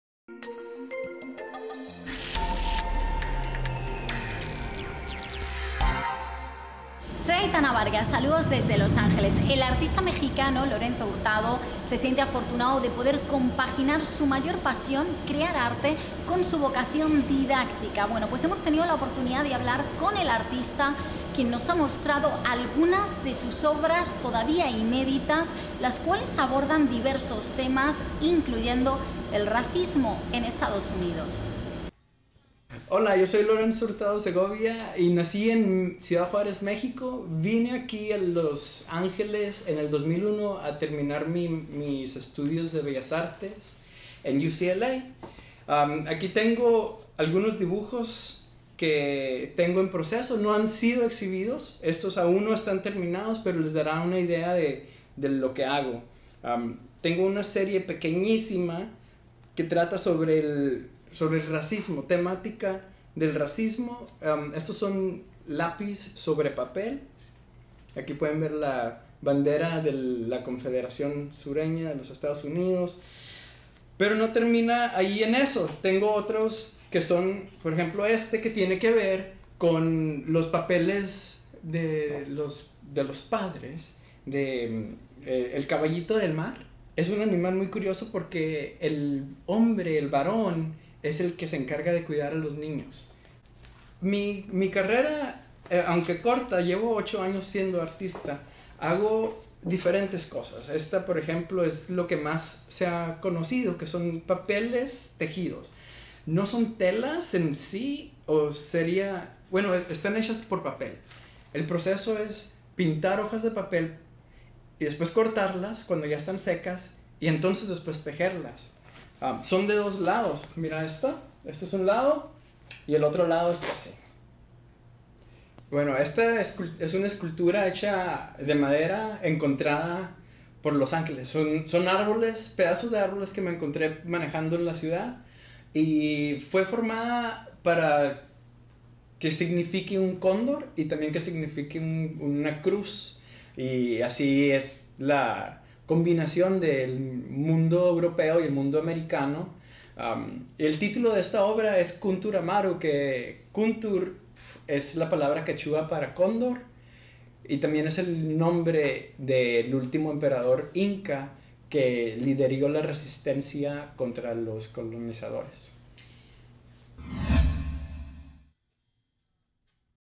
El mexicano que usa el arte para denunciar el racismo en EEUU
El joven artista plástico Lorenzo Hurtado utiliza su talento para denunciar el racismo en Estados Unidos y otros temas de interés.

Egresado de la Universidad de California en Los Ángeles (UCLA), expresa sus emociones y las del colectivo en esculturas y dibujos.

Es profesor del Otis College of Art & Design, en California, y trabaja en proyectos inéditos.